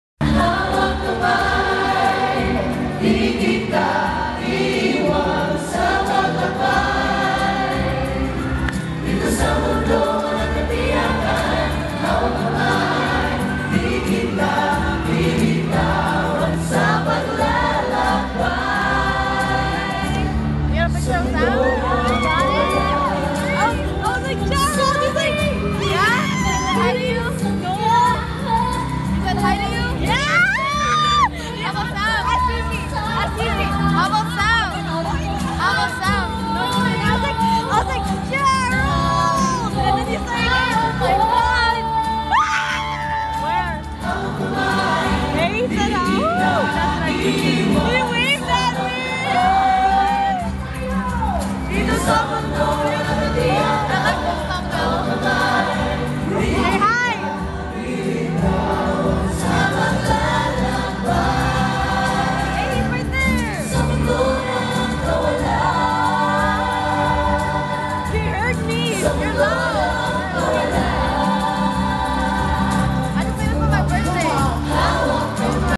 Star Magic Concert
we watched the concert in Ontario, California.
it was amazing to see all of them perform.
especially sam milby and gerald anderson